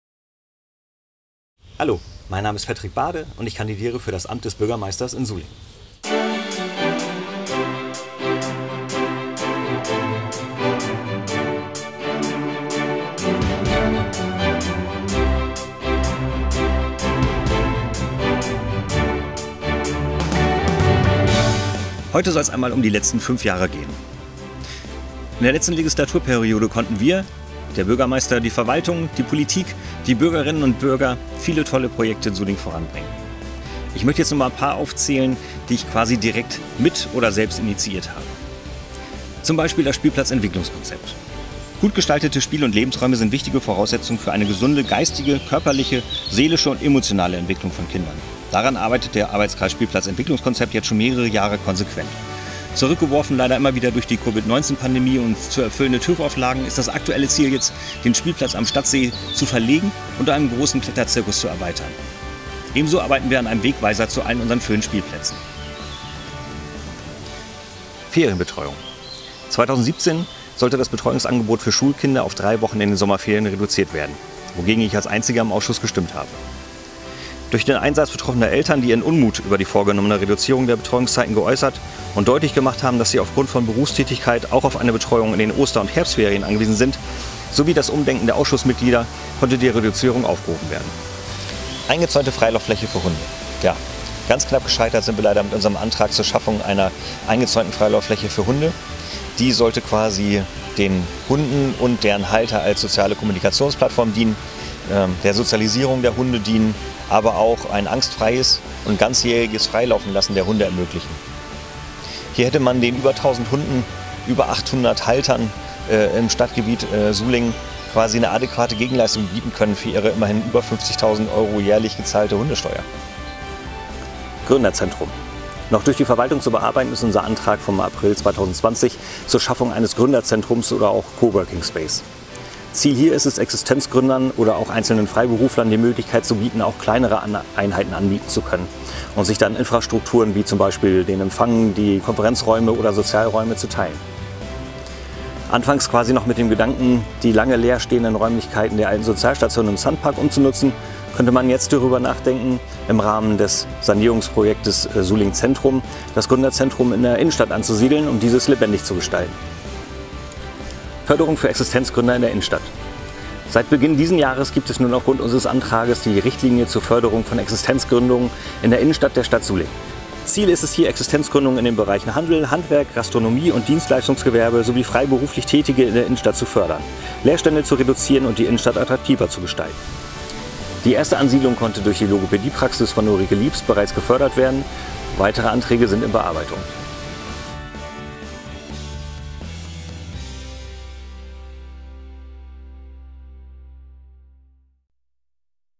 Das politische Wirken des Bürgermeisterkandidaten für Sulingen, Patrick Bade!
Liebe Sulinger*innen,
meine Name ist Patrick Bade und ich kandidiere für das Amt des Bürgermeisters in meiner Heimatstadt Sulingen!

In diesem Video möchte ich Ihnen heute etwas über mein politisches Wirken in den vergangenen 5 Jahren erzählen.

Bitte gehen Sie am 12. September 2021 wählen!